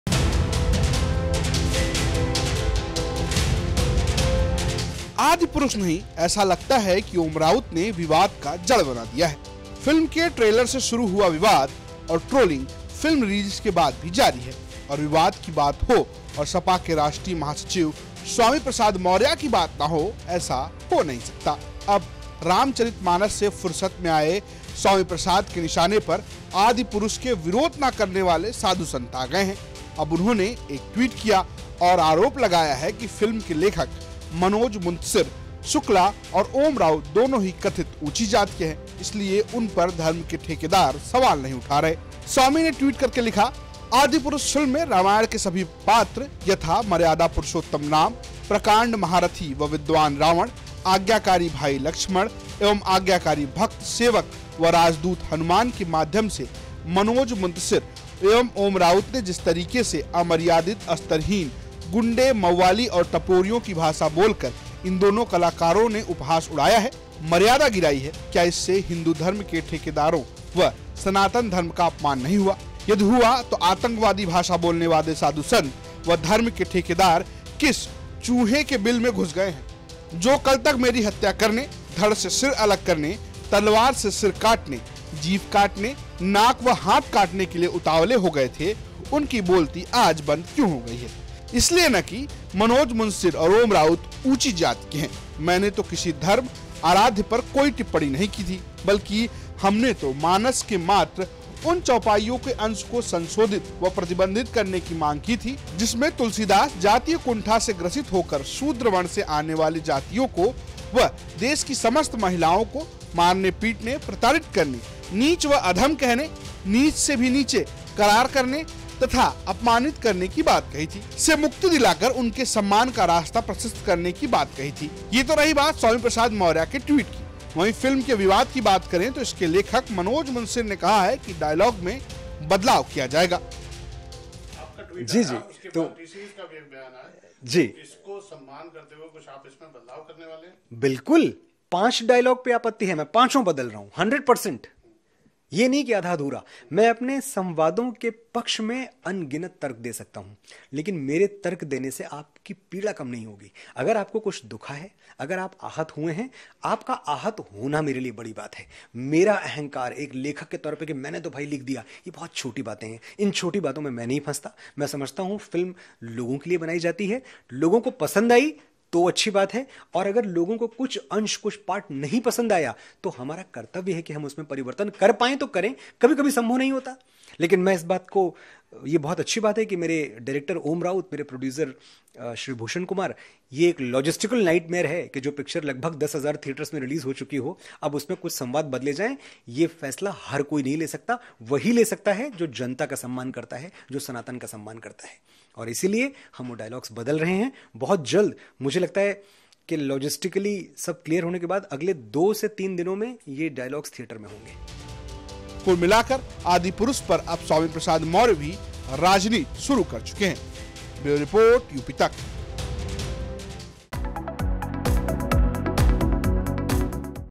Adipurush Controversy: अब स्वामी प्रसाद मौर्या ने खोल दिया मोर्चा, निकाल लाए जाति एंगल!
Adipurush Controversy: आदिपुरुष नहीं, ऐसा लगाता हैं कि ओम राउत ने विवाद का जड़ बना दिया है. फ़िल्म के ट्रेलर शुरू हुआ विवाद और ट्रोलिंग फ़िल्म रिलीज़ के बाद भी जारी है. और विवाद की बात हो और सपा के राष्ट्रीय महसचिव स्वामी प्रसाद मौर्या की बात ना हों, ऐसा हो नहीं सकता है. अब रामचरित मानस से फुर्सत में आए स्वामी प्रसाद के निशाने पर आदिपुरुष के विरोध ना करने वाले साधु-संत आ गए हैं. अब उन्होंने एक ट्वीट किया और आरोप लगाया है कि फ़िल्म के लेखक मनोज मुन्तसिर शुक्ला और ओम राऊत दोनों ही कथित ऊंची जाति के हैं, इसलिए उन पर धर्म के ठेकेदार सवाल नहीं उठा रहे हैं. स्वामी ने ट्वीट करके लिखा- 'आदिपुरुष फिल्म में रामायण के सभी पात्र यथा मर्यादा पुरुषोत्तम राम, प्रकांड महारथी व विद्वान रावण, आज्ञाकारी भाई लक्ष्मण एवं आज्ञाकारी भक्त, सेवक व राजदूत हनुमान के माध्यम से मनोज मुन्तसिर एवं ओम राऊत ने जिस तरीके से अमर्यादित स्तरहीन गुंडे, मवाली व टपोरियों की भाषा बोलवाकर इन दोनों कलाकारों ने उपहास उड़ाया है, मर्यादा गिराई है क्या इससे हिन्दू धर्म के ठेकेदारों व सनातन धर्म का अपमान नहीं हुआ? यदि हुआ तो आतंकवादी भाषा बोलने साधु-संत व धर्म के ठेकेदार किस चूहें के बिल में घुस गए हैं, जो कल तक मेरी हत्या करने, धड़ से सिर अलग करने, तलवार से सिर काटने, जीभ काटने, नाक-कान व हाथ काटने के लिए उतावले हों गए थे, उनकी बोलती आज बंद क्यों है? इसलिए न की मनोज मुन्तसिर और ओम राऊत ऊँची जाती के हैं। मैंने तो किसी धर्म, आराध्य पर कोई टिप्पणी भी नहीं की थी बल्कि हमने तो मानस के मात्र उन चौपाइयों के अंश को संशोधित व प्रतिबंधित करने की मांग की थी जिसमे तुलसीदास जातीय कुंठा से ग्रसित होकर शूद्र वर्ण में आने वाली जातियों को व देश की समस्त महिलाओ को मारने-पीटने, प्रताड़ित करने, नीच व अधम कहने, नीच से भी नीच करार करने तथा अपमानित करने की बात कही थी, से मुक्ति दिलाकर उनके सम्मान का रास्ता प्रशस्त करने की बात की थी।'वहीं, अगर फ़िल्म के विवाद की बात करें, तो इसके लेखक मनोज मुंतिशर ने कहा डॉयलॉग्स में बदलाव किया जाएगा.